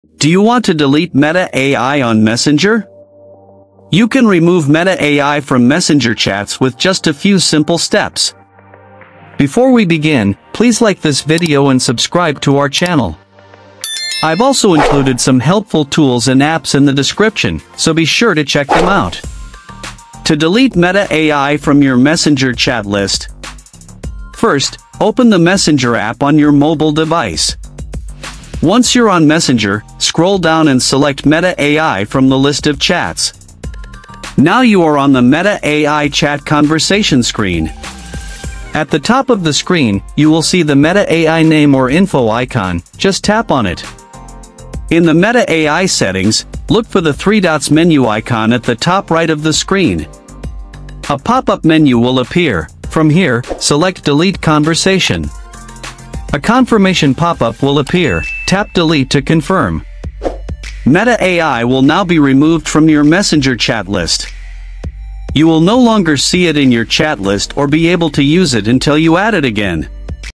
How To Delete Meta AI On Messenger | Remove Meta AI From Messenger
Do you want to delete Meta AI on Messenger? You can remove Meta AI from Messenger chats with just a few simple steps.

Deleting or removing Meta AI from Messenger means clearing the Meta AI chat from your chat list so it no longer appears among your conversations.

This does not fully disable Meta AI from Messenger globally (because it's built into the app), but it removes the AI chat so you won’t see it unless you open it again later.

If you don’t use Meta AI often, it can feel like an extra chat taking up space.

Deleting it helps declutter your Messenger inbox and keep only your active conversations visible.

Deleting Meta AI removes the AI chat, so you aren’t accidentally opening it or receiving prompts from it.

🔶STEPS🔶
To delete Meta AI from your Messenger chat list,
1. Open the Messenger app on your mobile device.
2. Scroll down and select Meta AI from the list of chats.
3. At the top of the screen, tap on the Meta AI name or info icon.
4. In the Meta AI settings, tap on the three dots menu icon.
5. A pop-up menu will appear; select Delete conversation.
6. Tap Delete to confirm.

Meta AI will now be removed from your Messenger chat list.

You will no longer see it in your chat list or be able to use it until you add it again.

- - - - - - - Similar videos:- - - - - - - - -

This Video Includes:
○remove meta ai from messenger chat
○how to turn off AI on Messenger
○disable Meta AI on FB Messenger